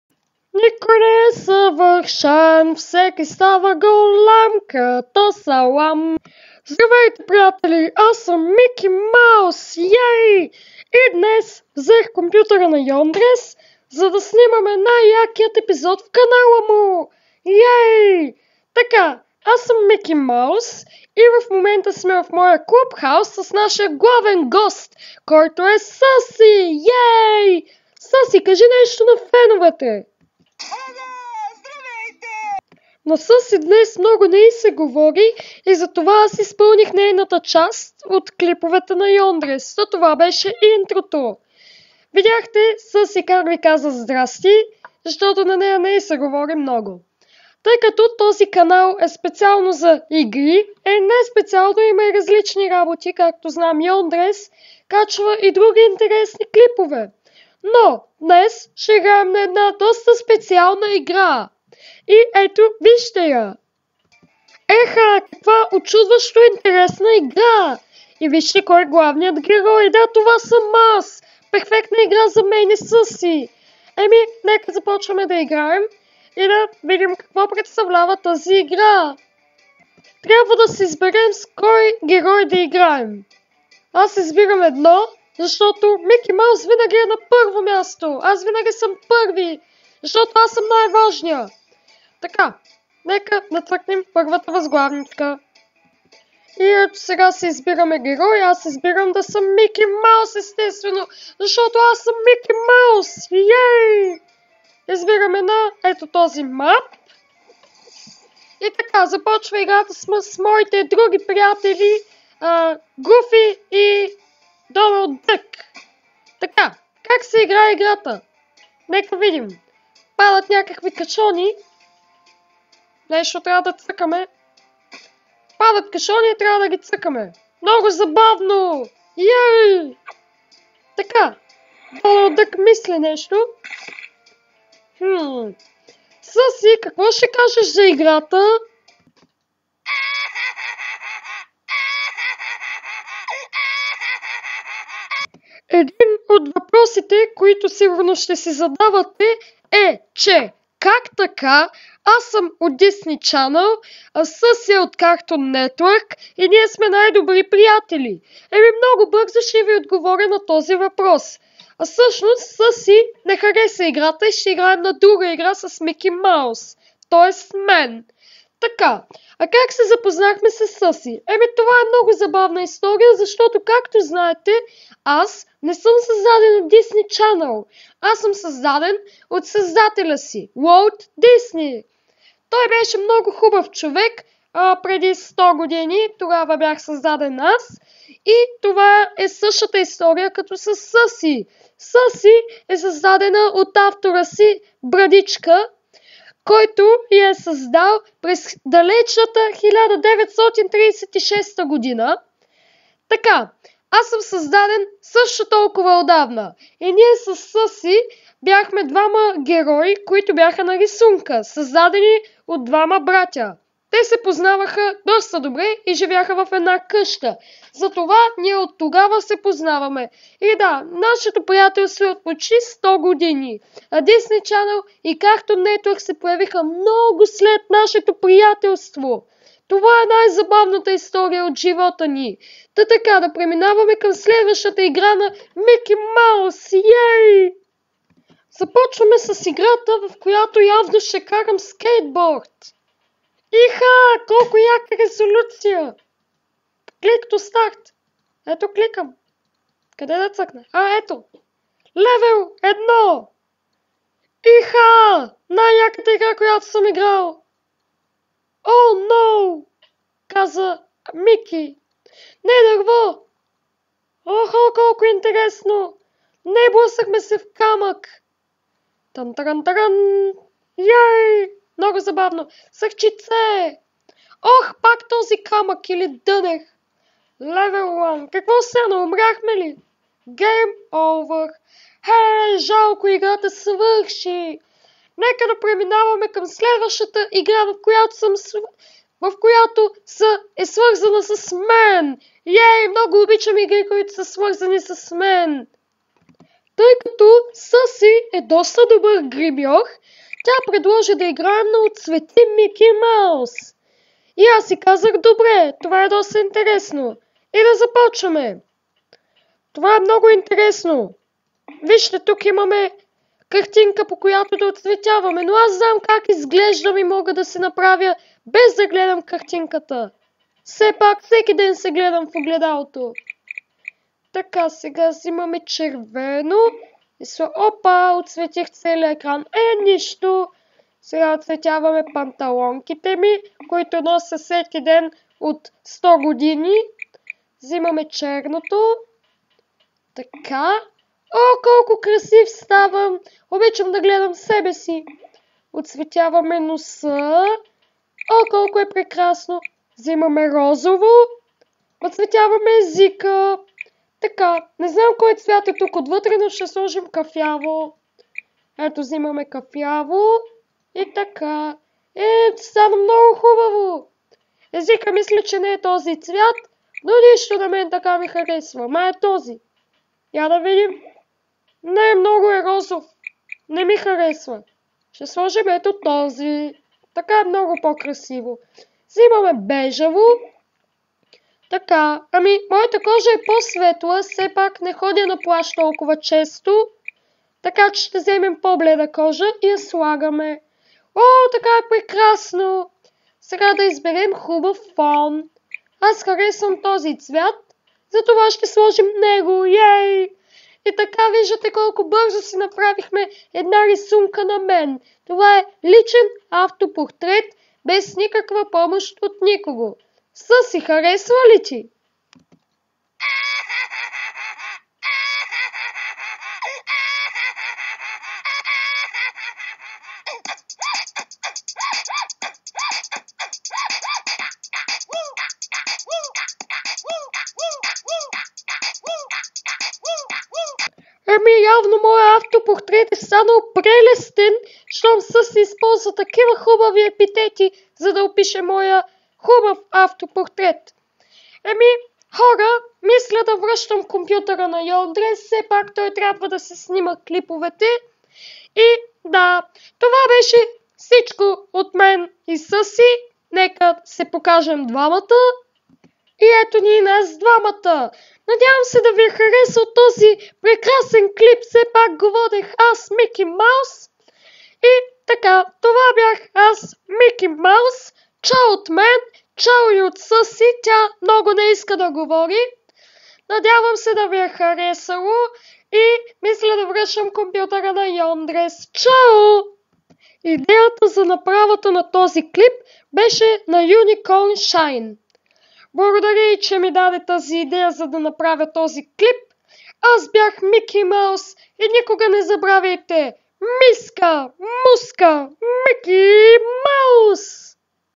Мики Маус и Съси vs Yondress- Кой прави по-хубави видеа?
аз бях Мики Маус а тук с мен беше Съси ако ви е  харесал този клип и искате още такива клипове в които взимам компютъра на Yondress не забравяйте да харесате видеото и да коментирате! :) МИСКА МУСКА МИКИИИ МАУС- ДО СКОРО